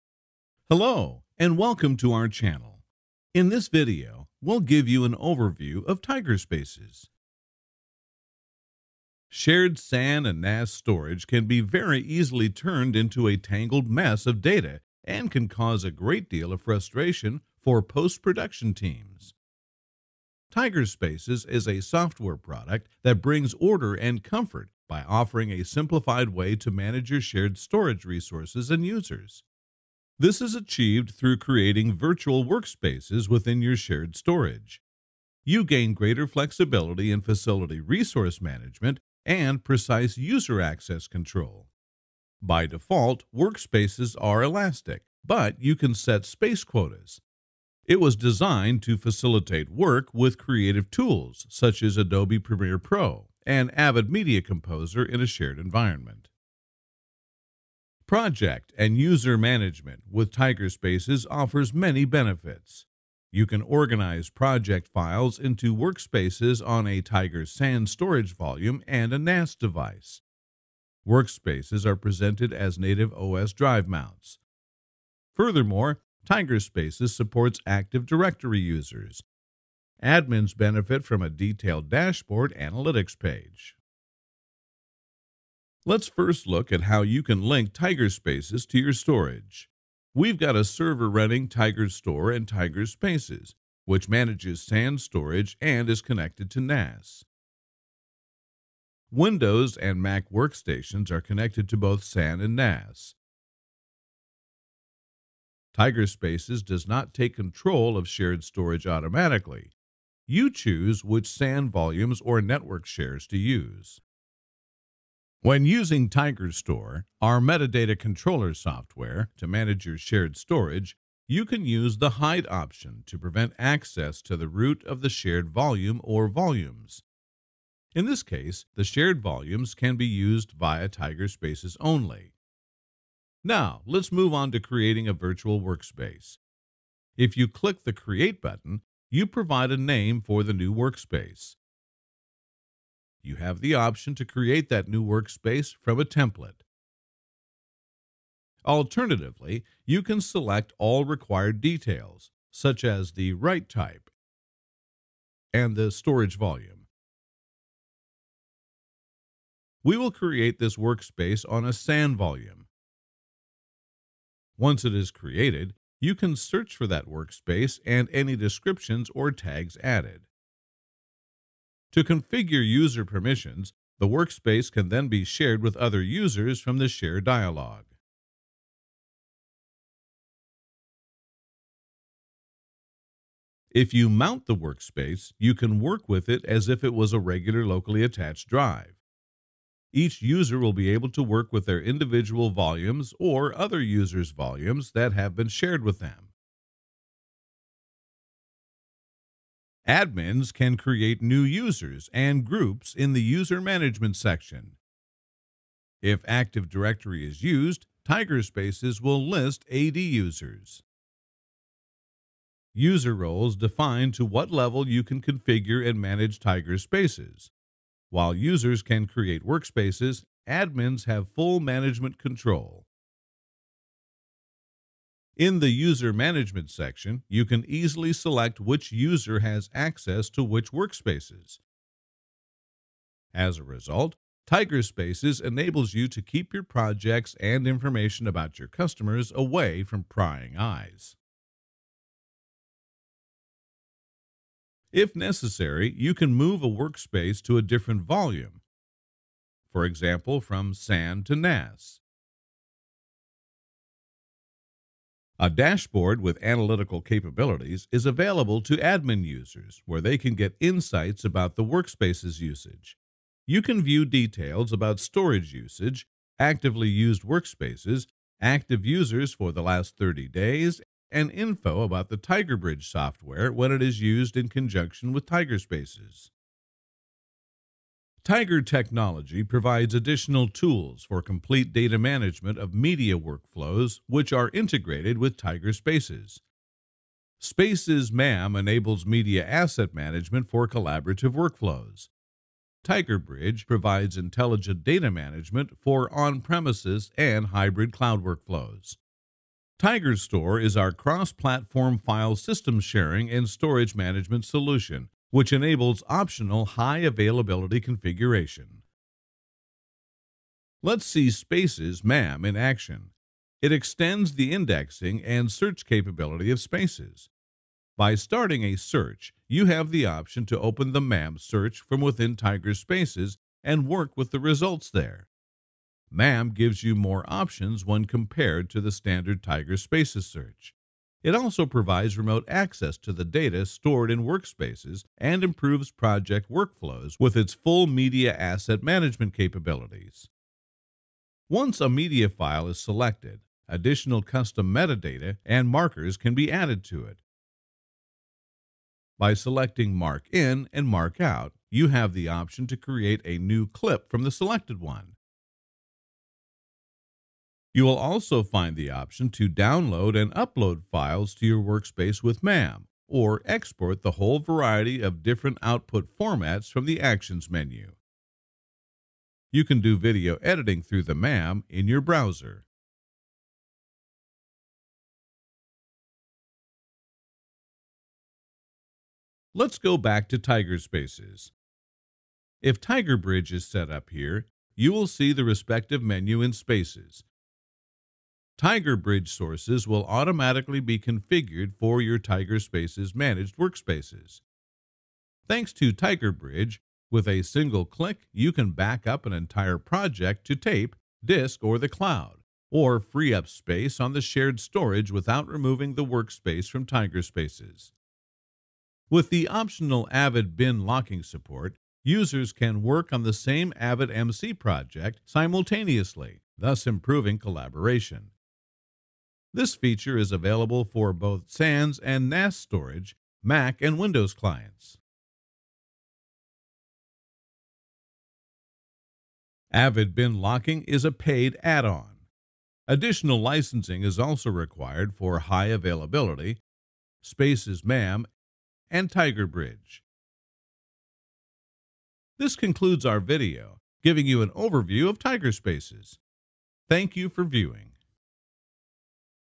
Tiger Spaces Quick Overview
Watch the video to find out how Tiger Spaces, our project & user management solution, can be used to simplify complex media workflows and enhance project management via virtual workspaces.